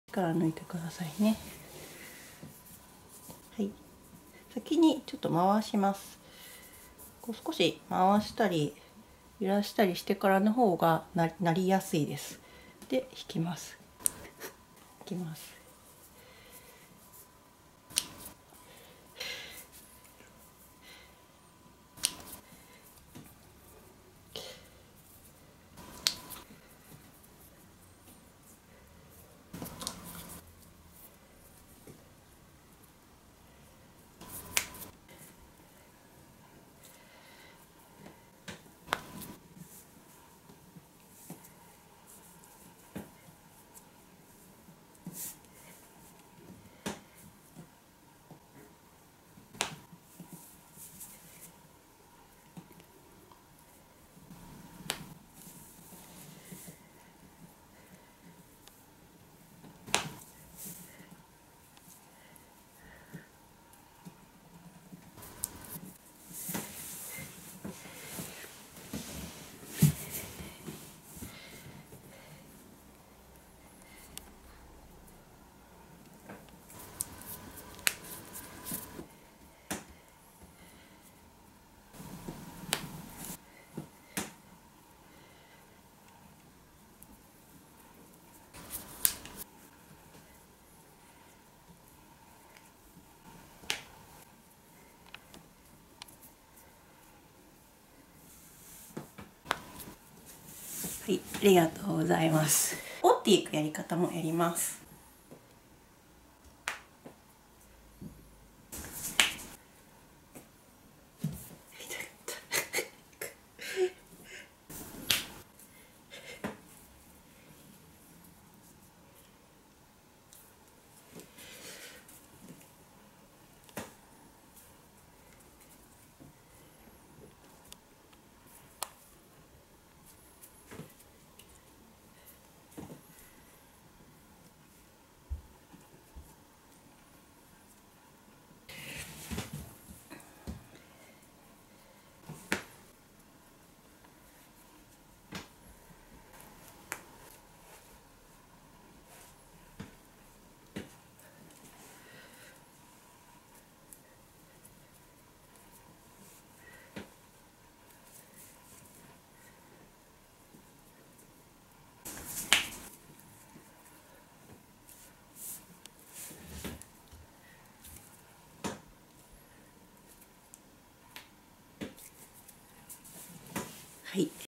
【指ポキ音】ＡＳＭＲ？夫の手足をポキポキ鳴らしてみた【りらく屋】
りらく屋ではカイロプラクティック（いわゆるボキボキ系整体）は行っておりません。
ただし、関節の調整や矯正を目的としてストレッチや牽引、押圧などを行ないます。その際に、音が出ることもあります。
無理に鳴らすことはしません。